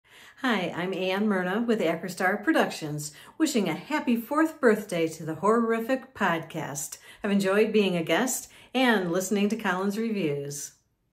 Ann Myrna wishes us a happy birthday!
Many thanks to Ann Myrna for the birthday wish! Check out Acrostar Films here

Keep it creepy. Keep it HORRORific!